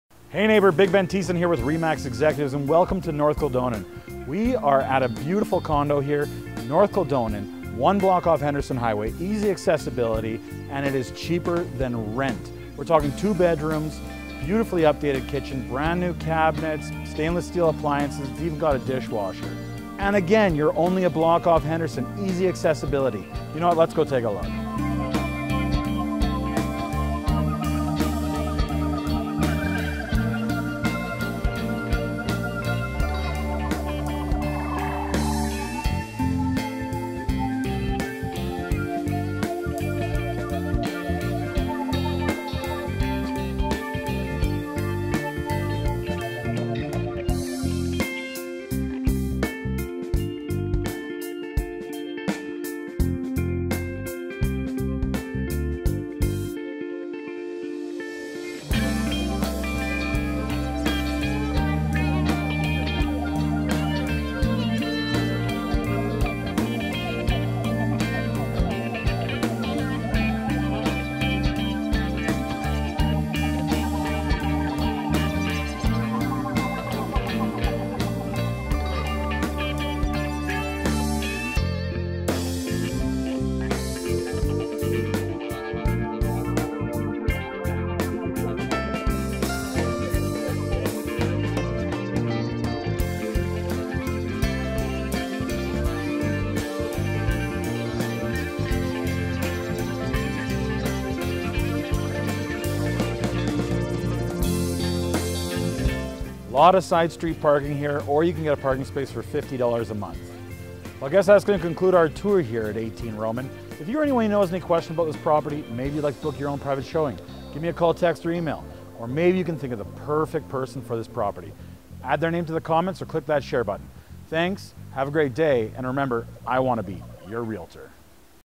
4-18 Roman - SOLD
SOLD! Only $129,900! Offers anytime! This beautiful corner ground level 2 bedroom condo offers a brand new eat-in kitchen complete with cabinets and stainless steel appliances (incl dishwasher), Large livingroom, two bedrooms and updated bathroom. New paint throughout and newer flooring. This condo is close to shopping and only a half block from Henderson for public transportation. Parking available. Book your showing today!